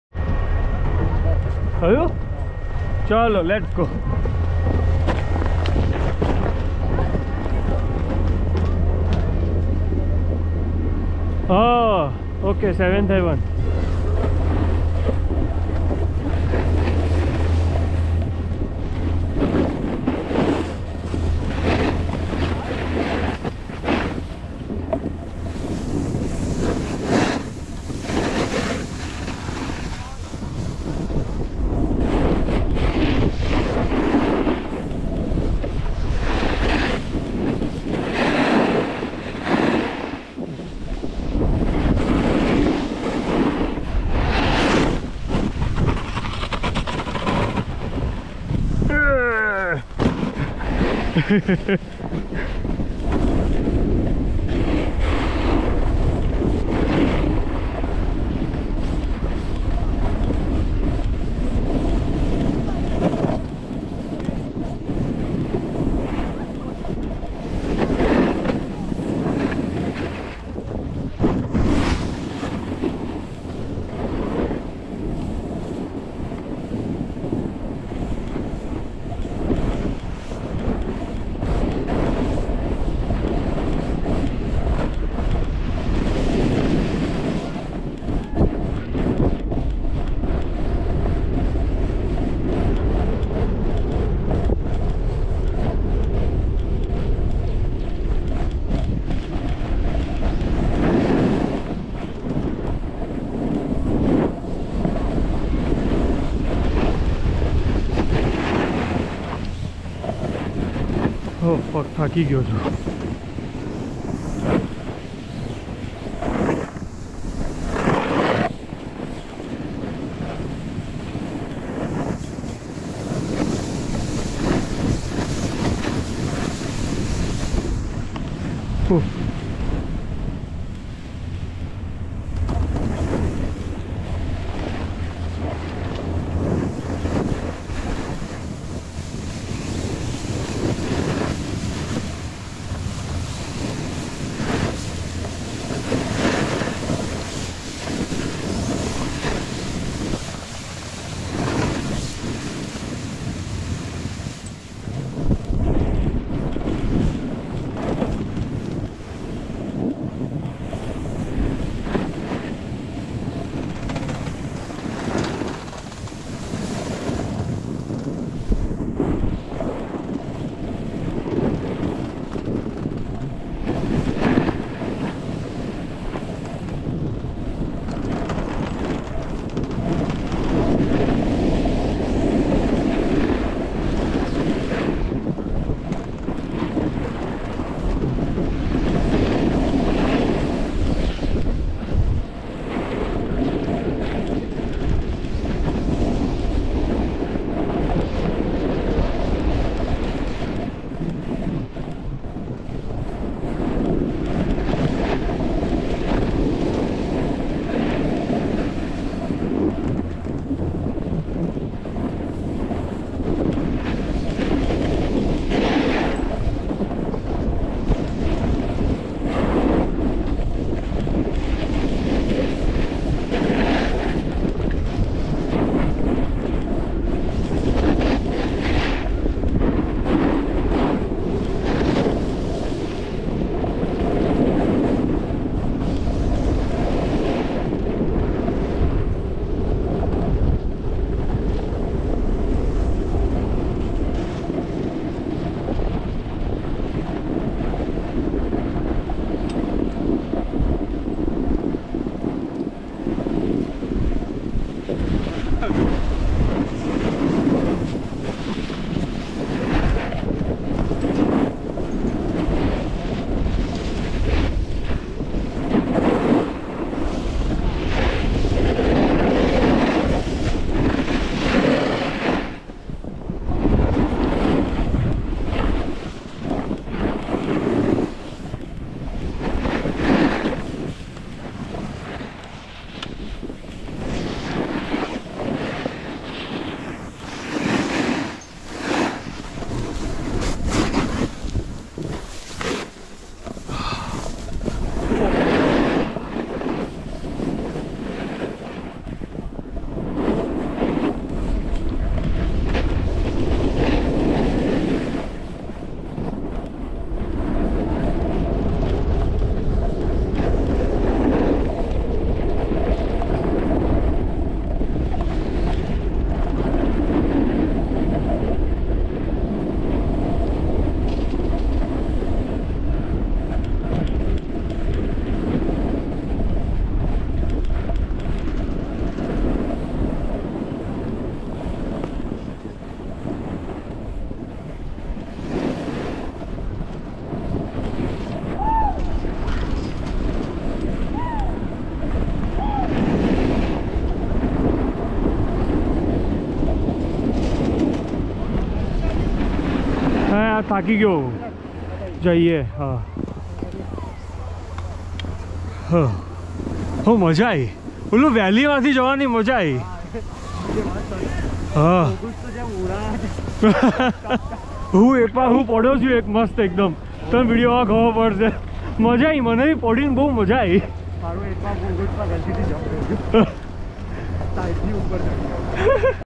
11th Feb 2023 - Stevens Pass - Skyline via Barrier Bowl - Blue run recorded
Skyline Express - Skyline - Barrier Bowl